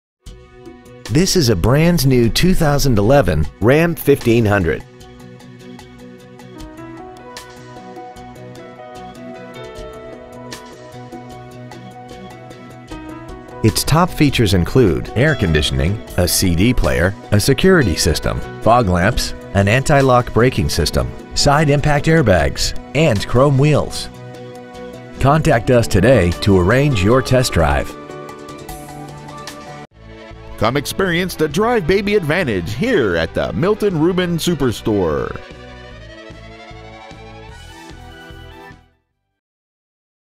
2011 Ram 1500 Columbia GA
Year : 2011
 Make : Ram
 Model : 1500
 Engine : 5.7L V8 HEMI MULTI-DISPLA
 Trans . : Automatic
 Exterior : Deep Cherry Red Crystal Pearl

 Interior : Dark Slate/Medium Graystone
 Stock : B2006

 2011 Ram 1500 Augusta GA